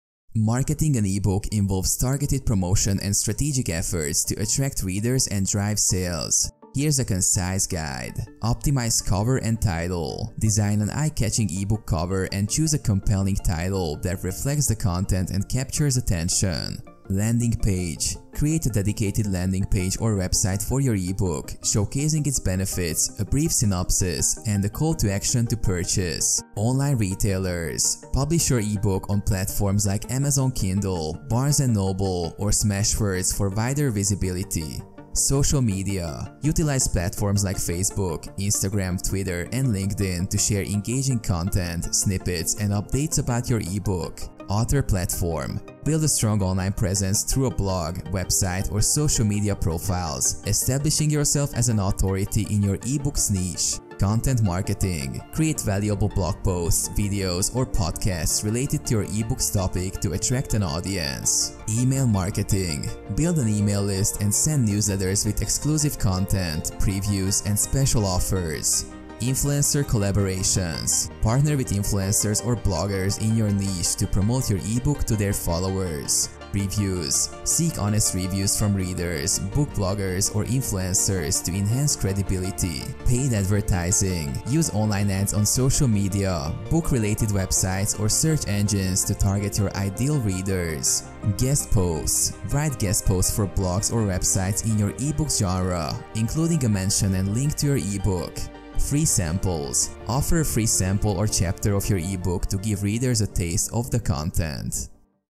How To Market An eBook In 2024?
In this video I will explain How You Can Market An eBook In 2024.